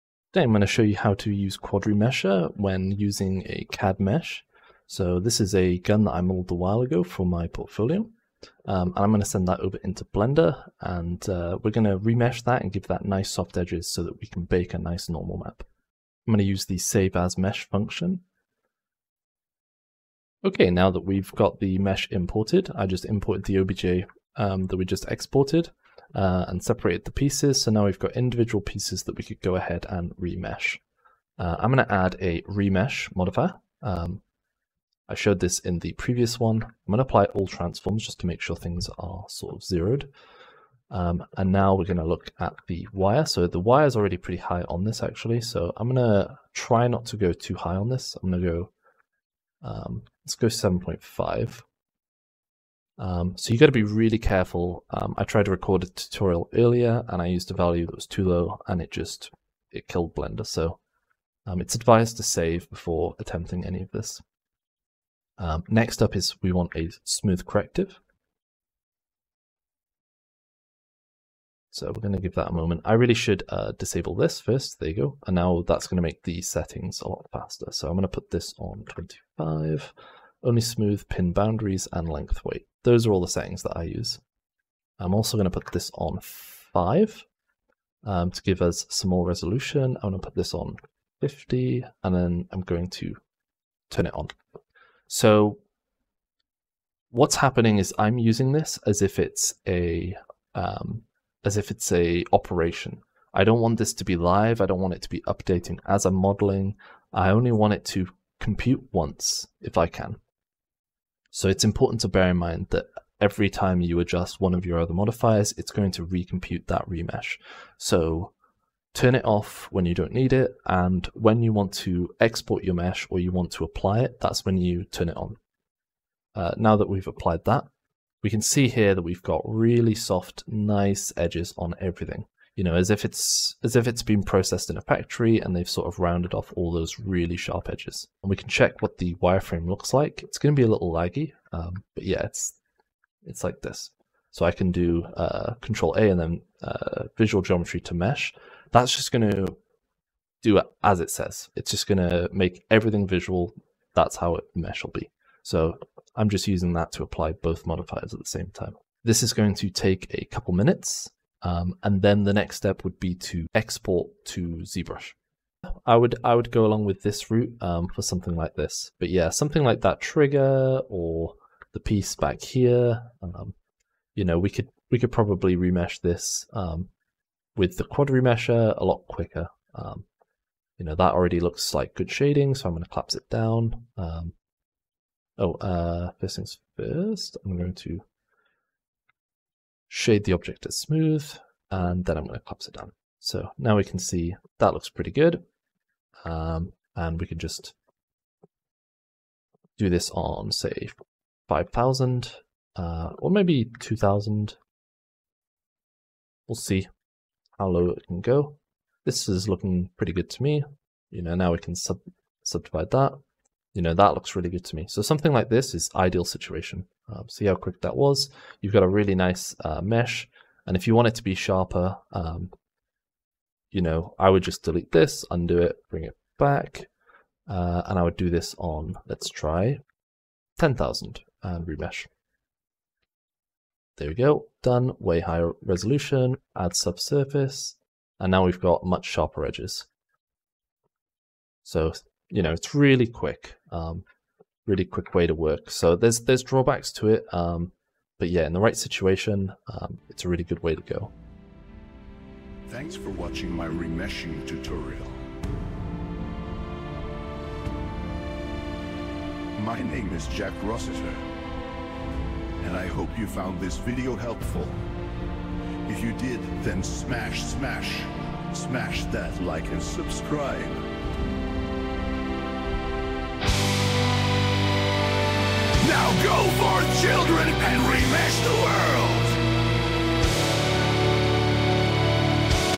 CAD to Blender. Hardsurface Workflow.
In this video I show how to create great and powerful 3d models. Use with caution.